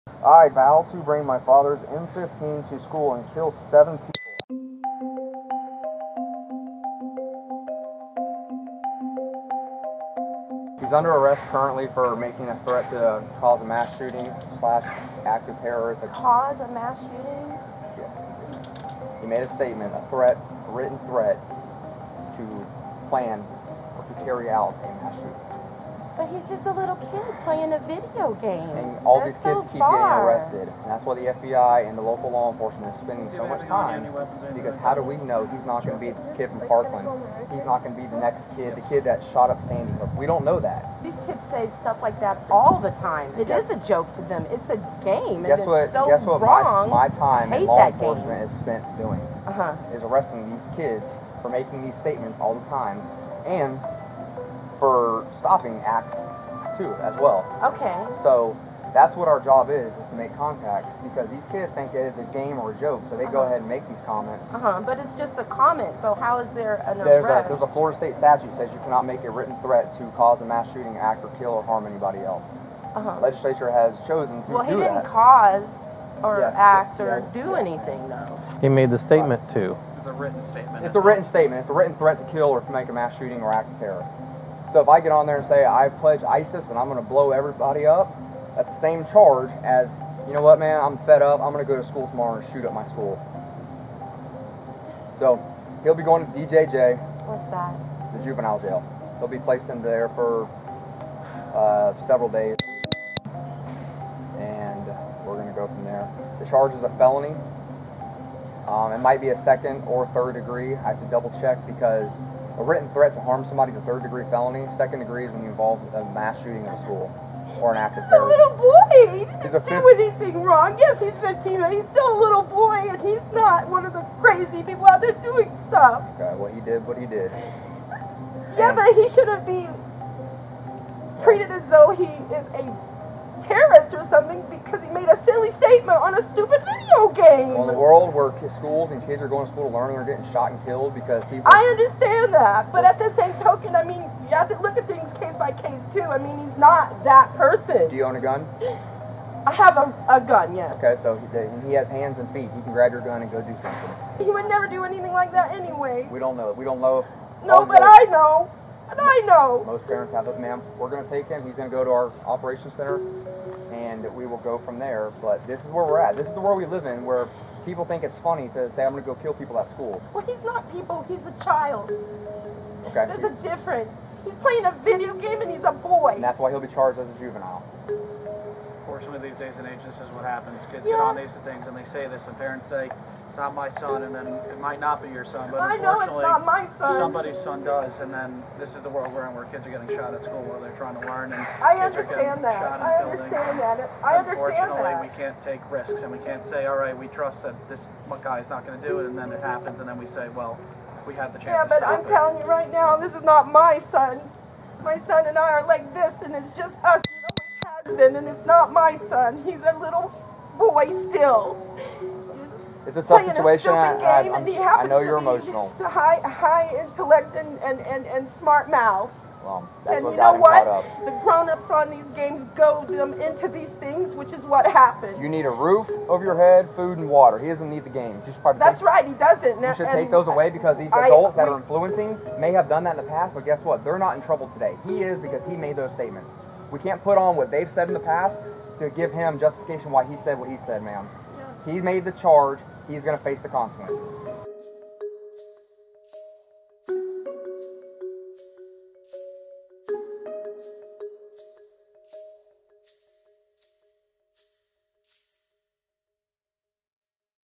Teen Arrested for 'Joking' About School Shooting in Video Game Chat | NowThis
‘How do we know he’s not gonna be the kid in Parkland?’ — This teen in Florida was arrested after ‘joking’ about shooting up his school in a video game chat.

In US news and current events today, a Florida teen in Volusia County was arrested by police after he made a mass shooting threat in a video game chat. In the police interaction caught on camera, they say the teen was only joking about bringing a gun to school, but police explained that they don't know he's not the next Parkland shooting suspect, who killed many at Marjory Stoneman Douglas High School, or the next Sandy Hook shooter. Many are saying enough is enough to gun violence in the United States.